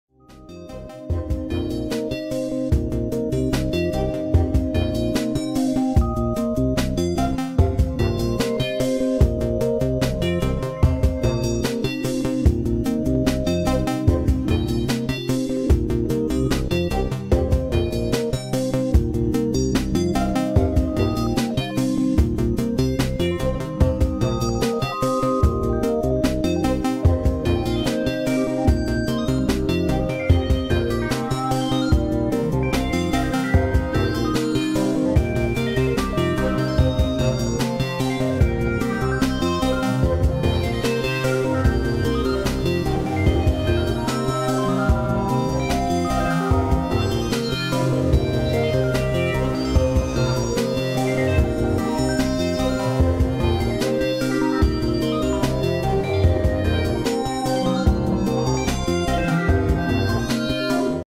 Metropolis Bass and Odessa
Video I tried to make. Model D on bass sequenced by Metropolis. Odessa modulating heavy in the background.